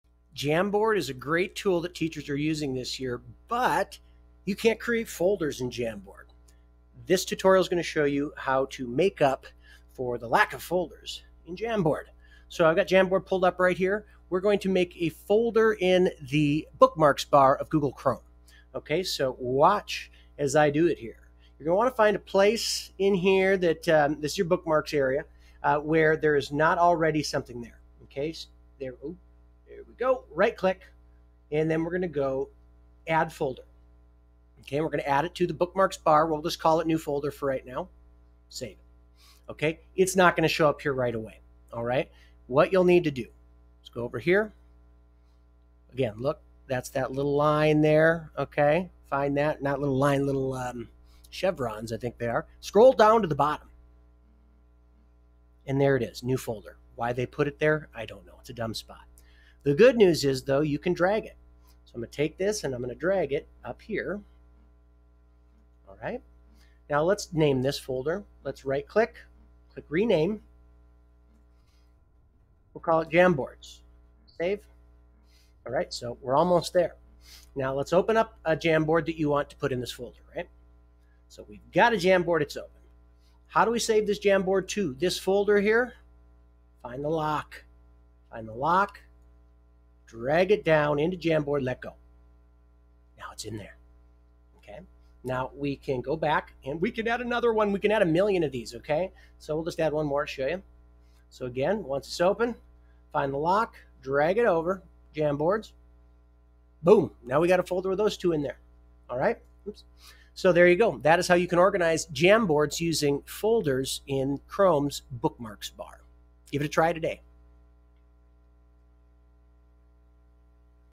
How to Organize Jamboards into a Folder using the Bookmarks Bar in Google Chrome
Jamboard doesn't allow you to organize your Jams by folder.  This video provides a solution to this problem.